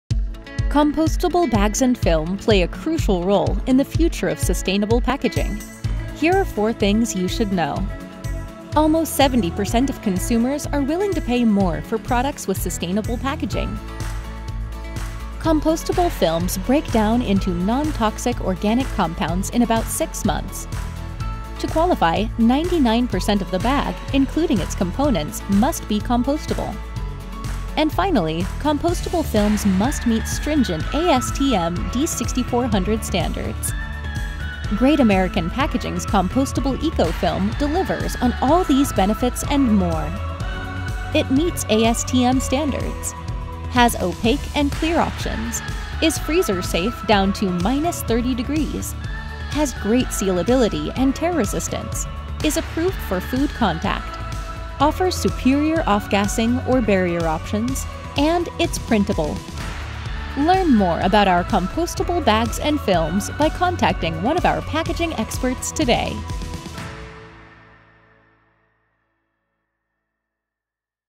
GAP Compostable Bags and Films
Compostable bags and films play a crucial role in the future of sustainable packaging.  Great American Packaging is proud to offer a new line of compostable products.